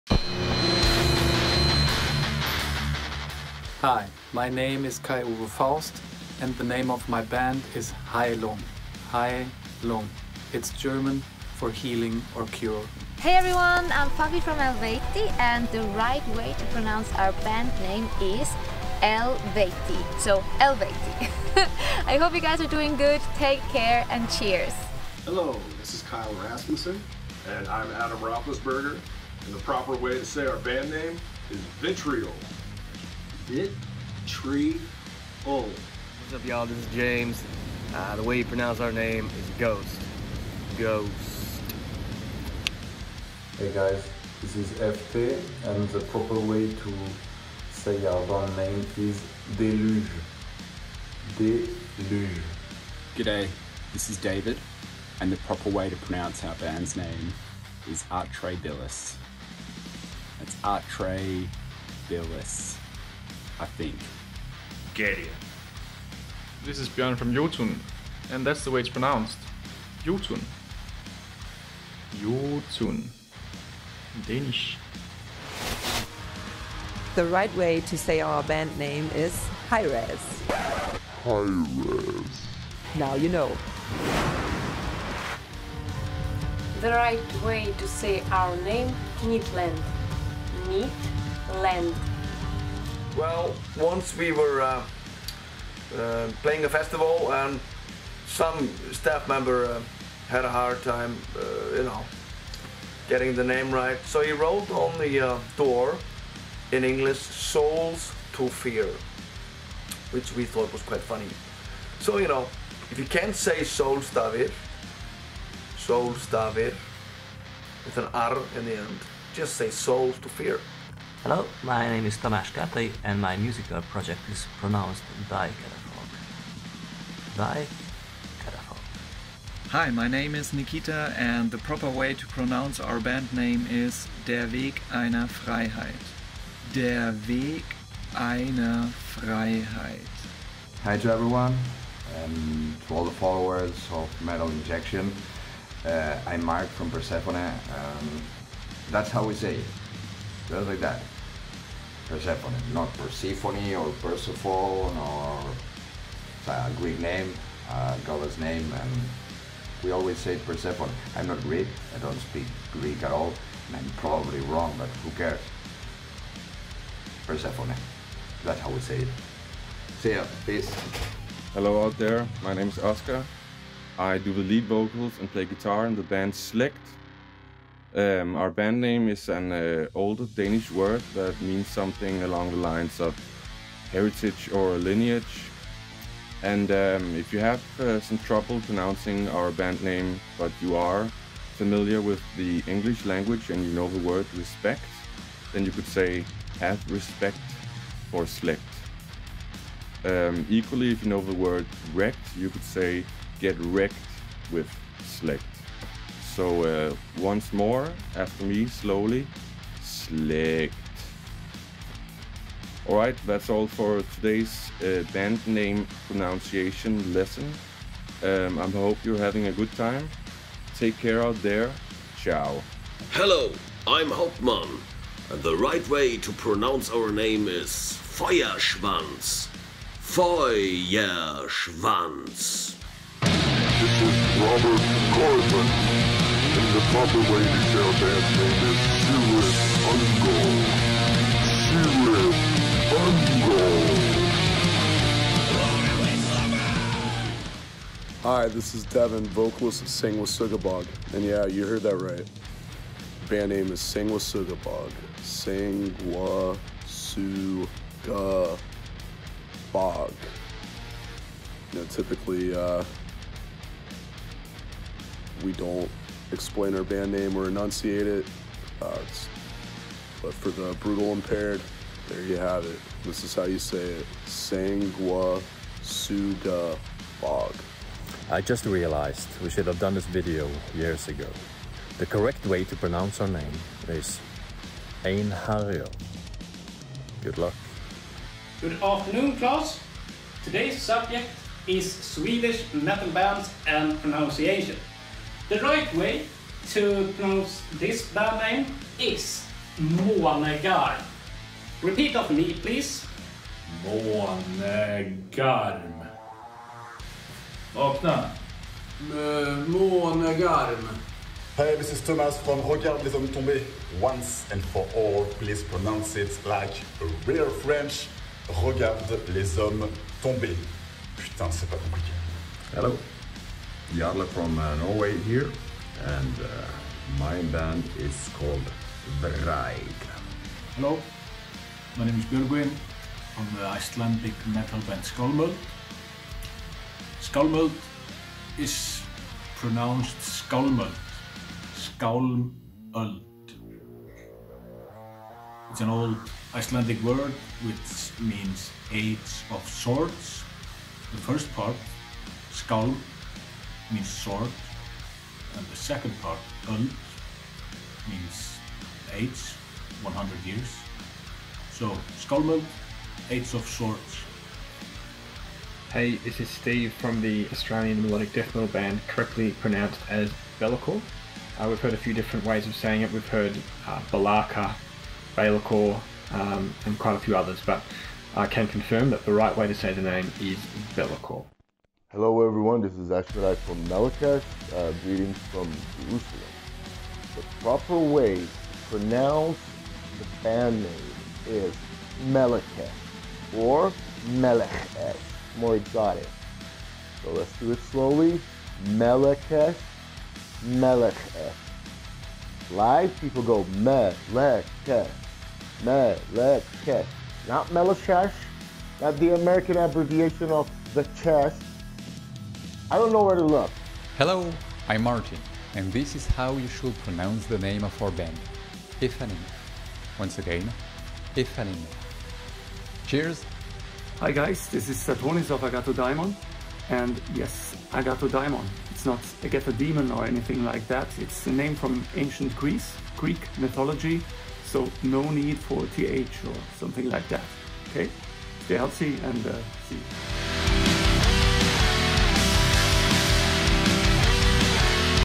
Proper Band Name Pronunciations By The Artist Themselves (Vol. 1) | Metal Injection
Have you ever wondered what is the right way to pronounce your favorite band names? Metal Injection has you covered as we ask the artists themselves to send a quick tip on the proper pronunciation for our sake, and theirs!

00:00 Heilung
00:13 Eluveitie
00:25 Vitriol
00:36 Gost
00:44 Deluge
00:53 Atrae Bilis
1:04 Gaerea
1:06 Iotunn
1:16 Hiraes
1:25 Nytt Land
1:33 Solstafir
2:00 Thy Catafalque
2:10 Der Weg Einer Freihiet
2:20 Persefone
2:52 Slaegt
3:44 Feuerschwanz
3:52 Cirith Ungol
4:08 Sanguisugabogg
4:39 Einherjer
4:49 Månegarm
5:12 Regarde Les Hommes Tomber
5:23 Vried
5:31 Skálmöld
6:05 Be'lakor
6:22 Melechesh
6:55 Aephanemer
7:05 Agathodaimon